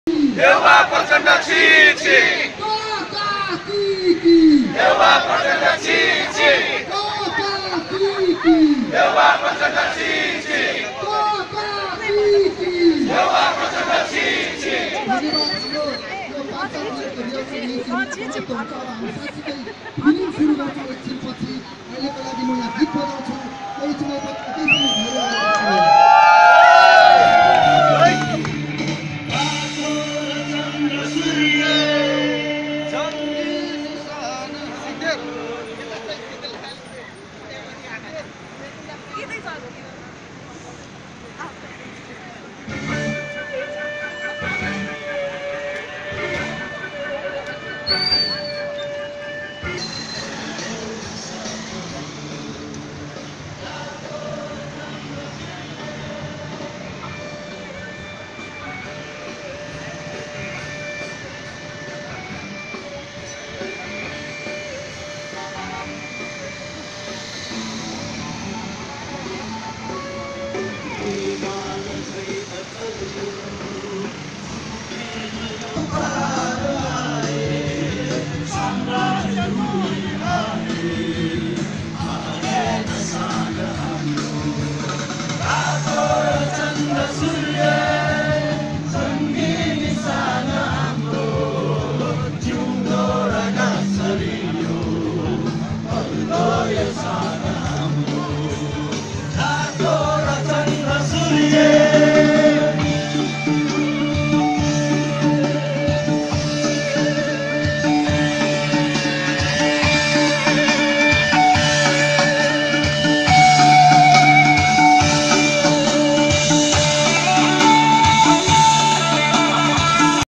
Protest against the impeachment motion of CJ Sushila Karki
Before the rally leaves Maitighar Mandala for New Baneshwor. People from different walks of life; students, people from civil society and few political parties (Bibeksheel Nepali, Sajha Party and Naya  Sakti) took part in the mass procession on Monday,  May 1, 2017.